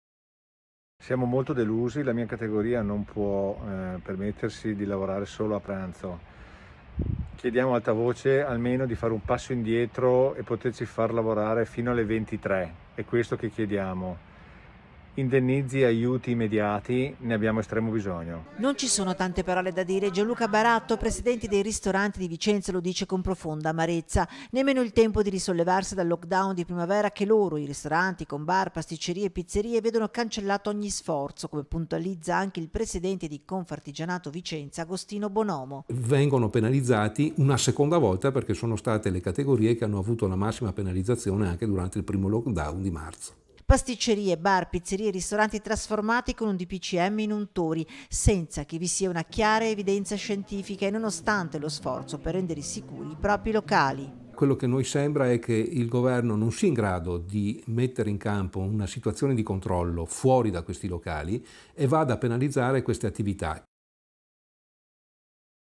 CONFARTIGIANATO; «GOVERNO INADEGUATO» | 26/10/2020
TG Vicenza 26/10/2020 - VICENZA - Contro il nuovo Dpcm si scaglia anche la Confartigianato, commercianti visti come untori. La delusione dei ristoratori.  || Non ci sono tante parole da dire e Gianluca Baratto presidente dei ristoratori di Vicenza le dice con profonda amarezza. Nemmeno il tempo di risollevarsi dal Lockdown di primavera che loro, i ristoranti con bar, pasticcerie e pizzerie vedono cancellato ogni sforzo come puntualizza anche il presidente di Confartigianato Vicenza Agostino Bonomo per il quale sono le stesse categorie che vengono penalizzate come a marzo. Pasticcerie, bar pizzerie e ristoranti trasformati con un Dpcm in untori senza che vi sia una chiara evidenza scientifica e nonostante lo sforzo per rendere sicuri i propri locali. Duro l’attacco al governo di confartigianato che parla di incapacità di sapere organizzare e gestire. - Intervistati GIANLUCA BARATTO (Presidente ristoratori Vicenza), AGOSTINO BONOMO (Presidente Confartigianato Vicenza-Veneto) (A cura di: Gabriella Basso) - Segui Rete Veneta anche sul digitale terrestre! Visita il sito